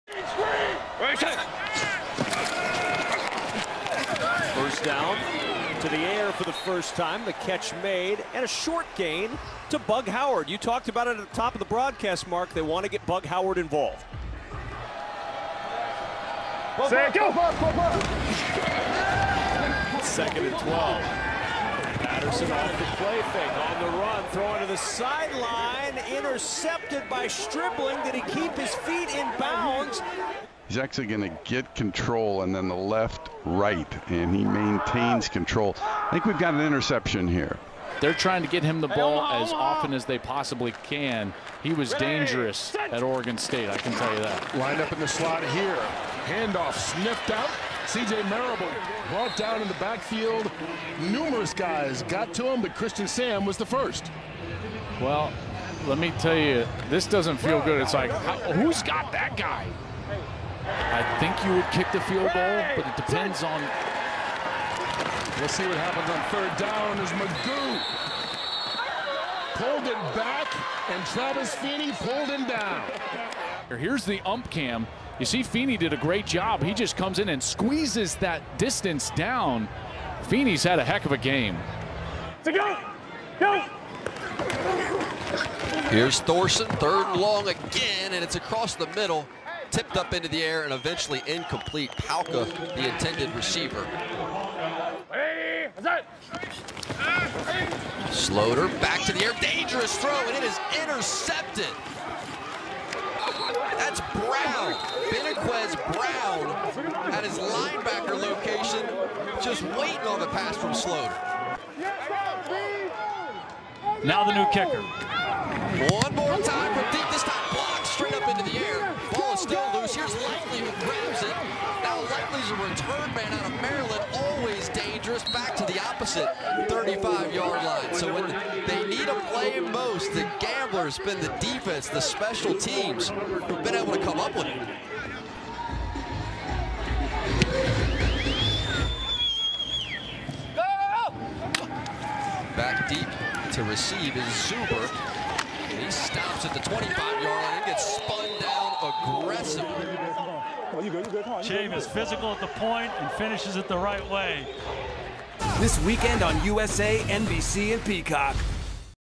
USFL Week 4: All the best helmet cam and ref cam shots | NBC Sports
Cameras on the headgear of both the players and referees gives you a one-of-a-kind look at all the action in the USFL -- here are the best shots from Week 4.

USFL Week 4L All the best helmet cam and ref cam shots | NBC Sports